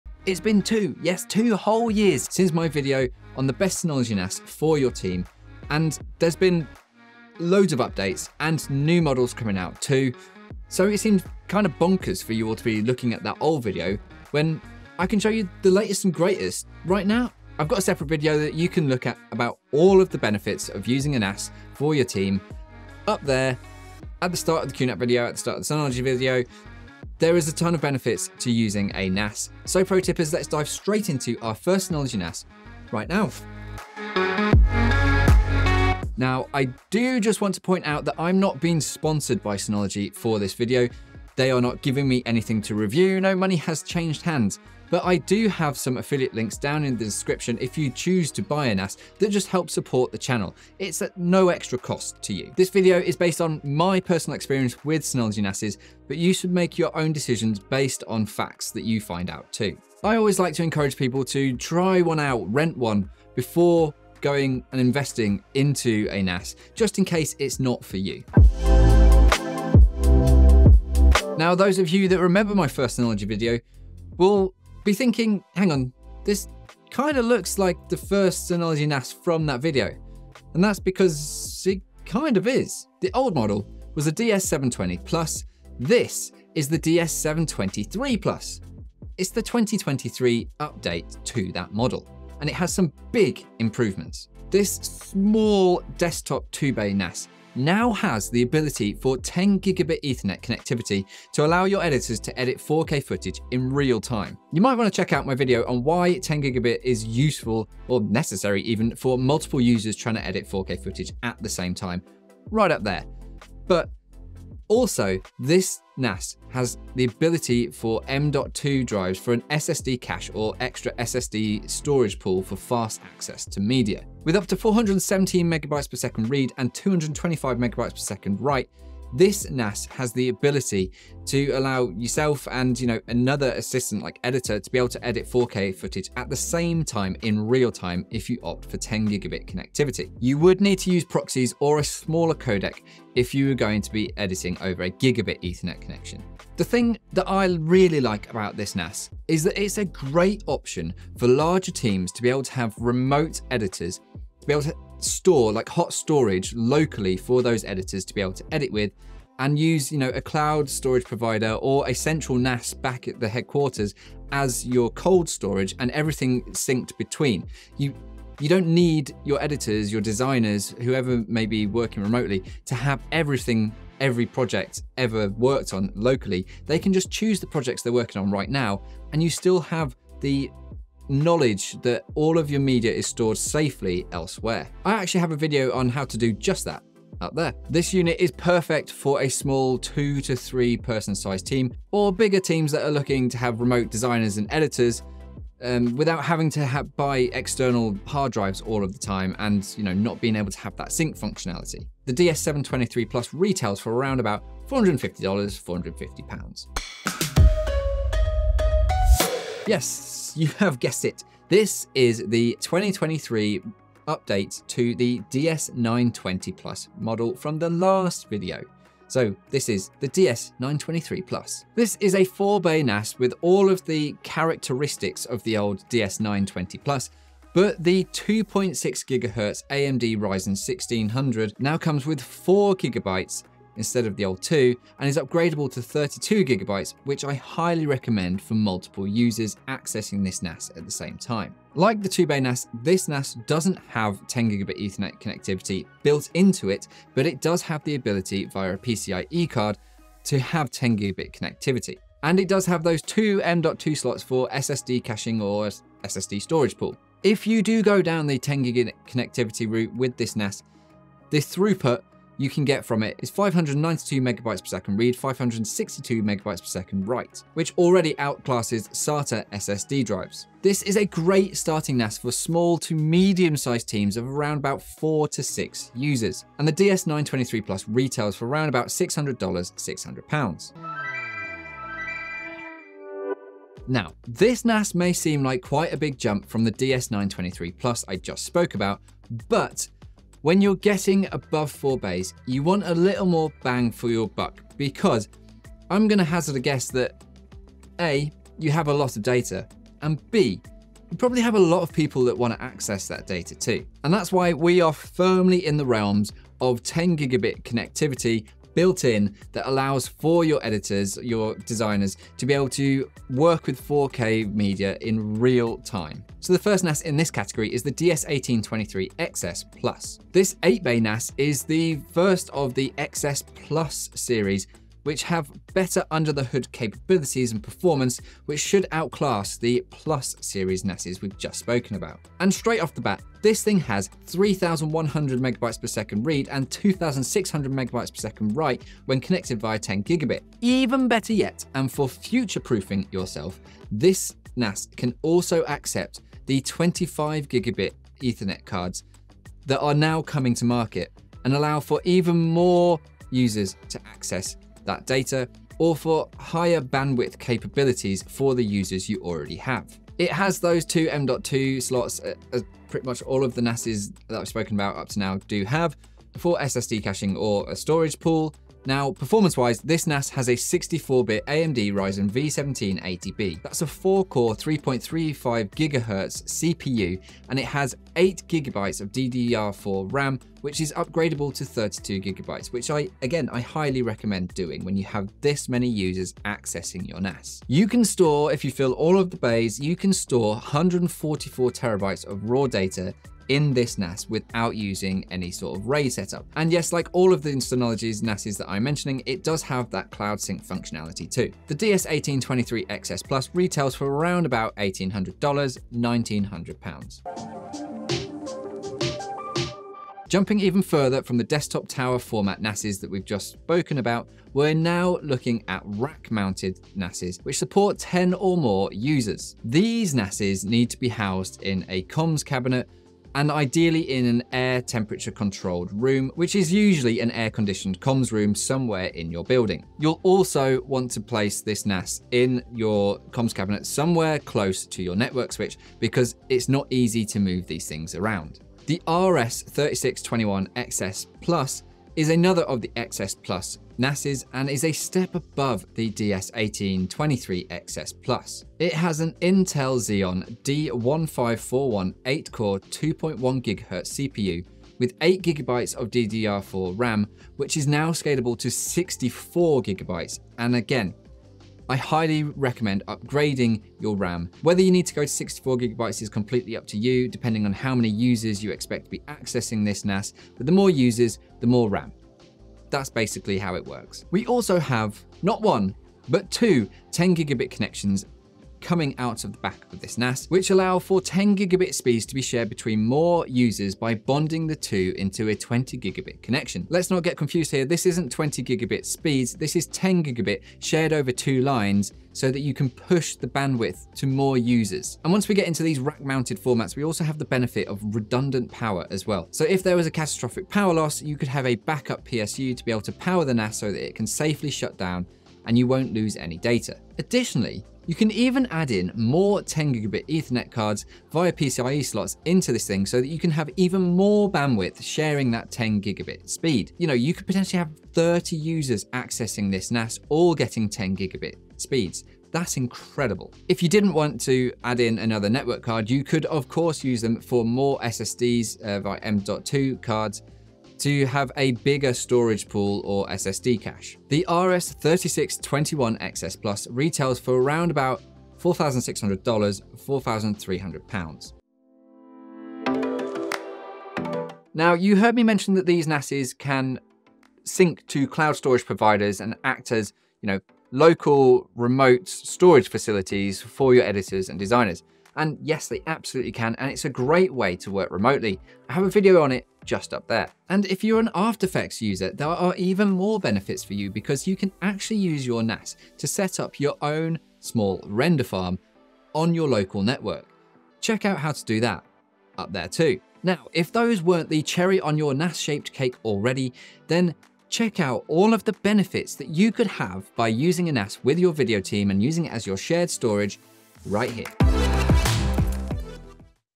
Which Synology is the Best NAS for Video Editing? 2025 Update!
It's been years since my video on Which Synology NAS is the Best for Video Editing for Your Team and there have been many updates and changes since then. Find out which is the best for you in 2023.

Skip Ahead:
00:00 - What's Up
01:09 - DS723+ 1-2 Users
03:27 - DS923+ 3-4 Users
04:41 - DS1823xs+ 4-10 Users
06:47 - RS3621xs+ 10+ Users
09:01 - Cloud Sync & Render Farm

DigiProTips is all about working smarter and not harder.